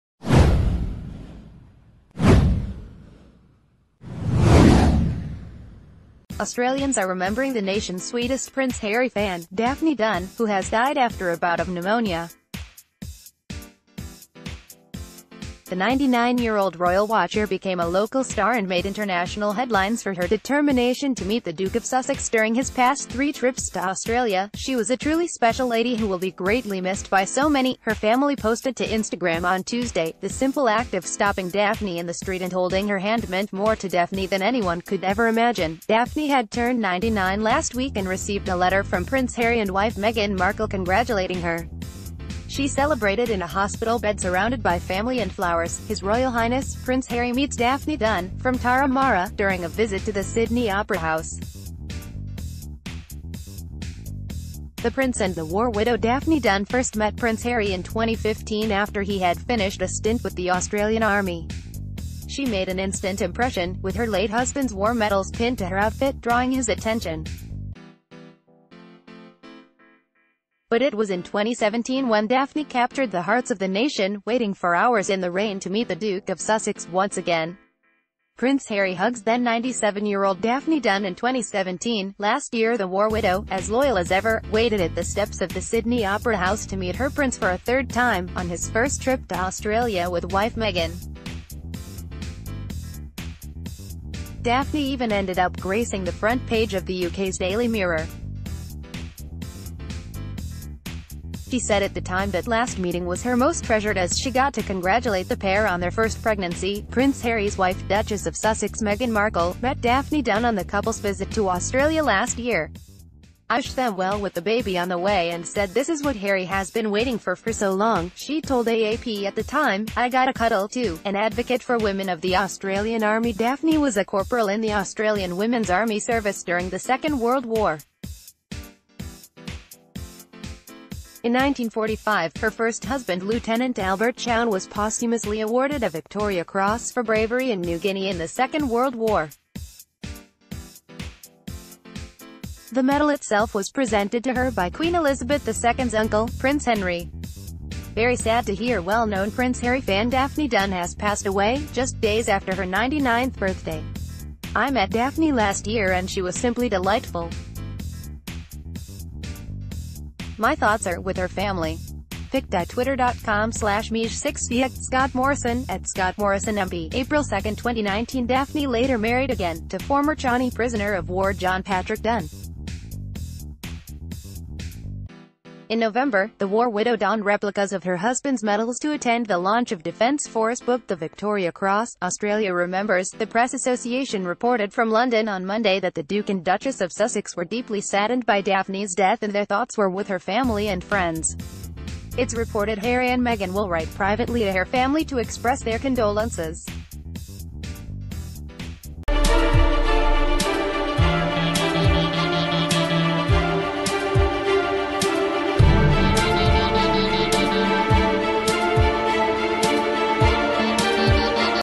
Prince Harry's biggest Aussie fan Daphne Dunne, dies at 99
Australians are remembering the nation's sweetest Prince Harry fan, Daphne Dunne, who has died after a bout of pneumonia. The 99-year-old royal watcher became a local star and made international headlines for her determination to meet the Duke of Sussex during his past three trips to Australia. "She was a truly special lady who will be greatly missed by so many," her family posted to Instagram on Tuesday."The simple act of stopping Daphne in the street and holding her hand meant more to Daphne t...